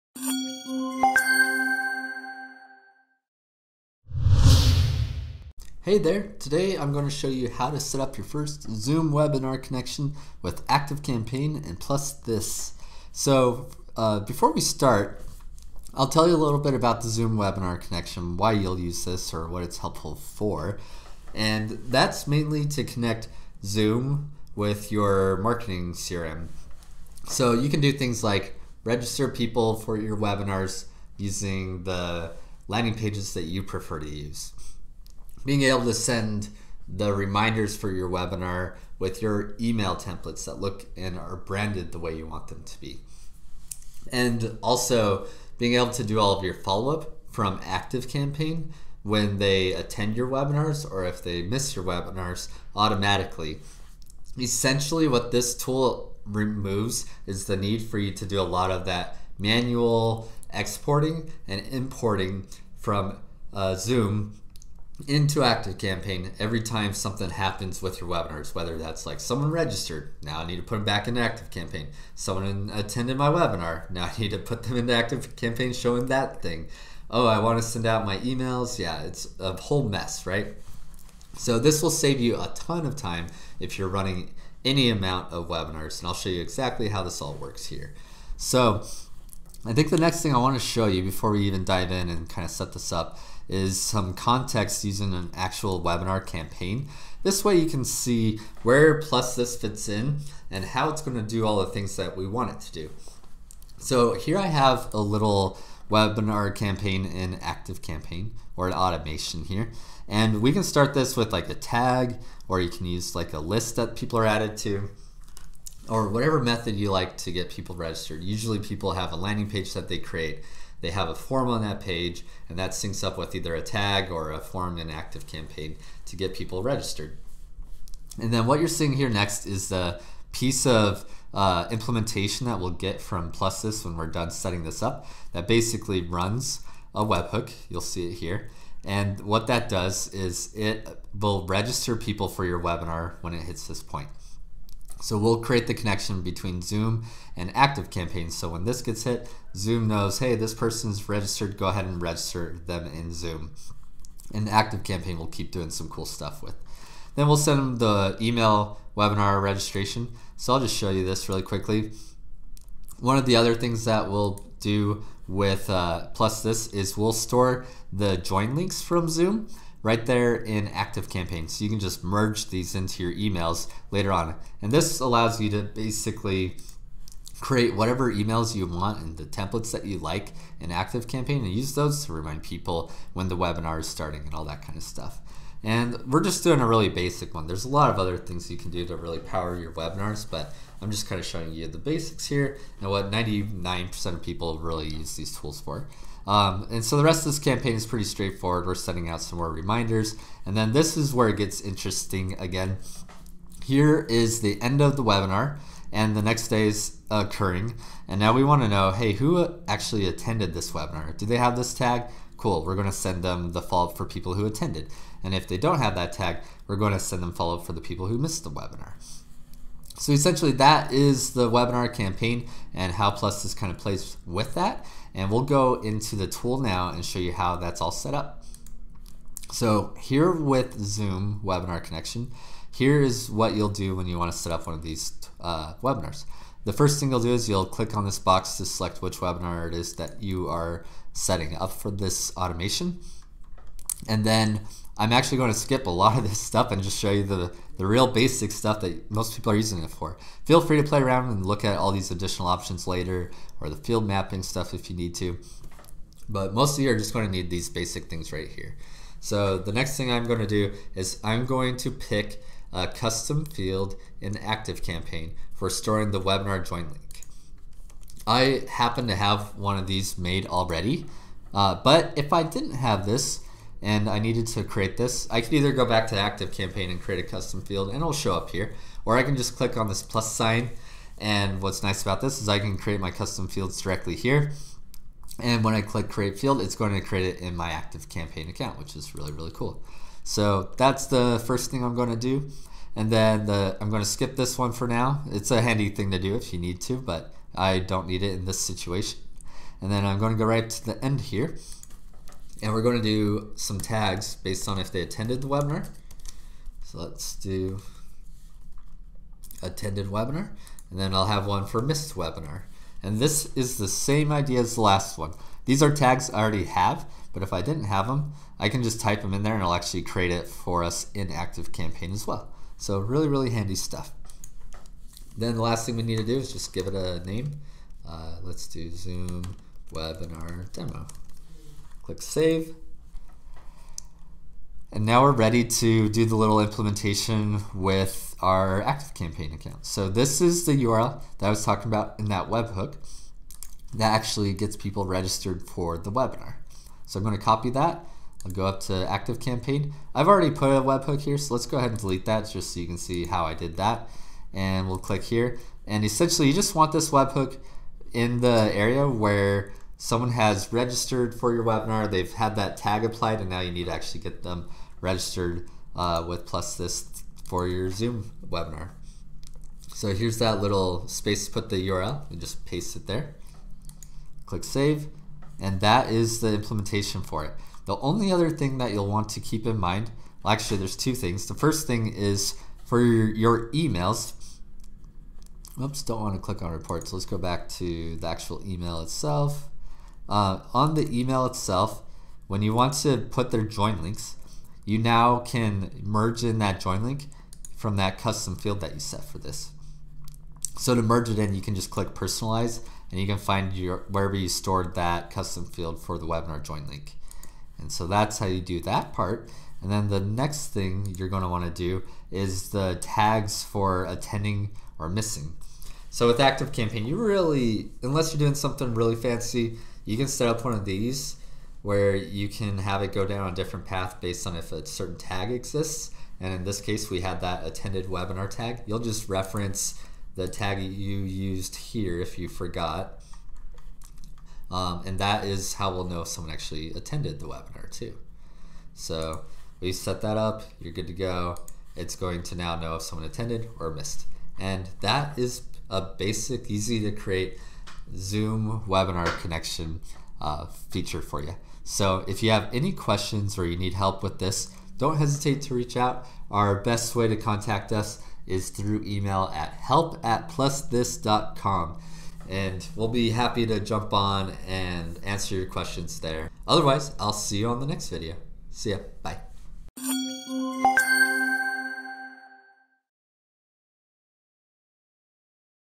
ActiveCampaign - Zoom Webinar Tool
Connect your Zoom account to your marketing platform. Register guests, send reminders, segment attendees and non-attendees and more.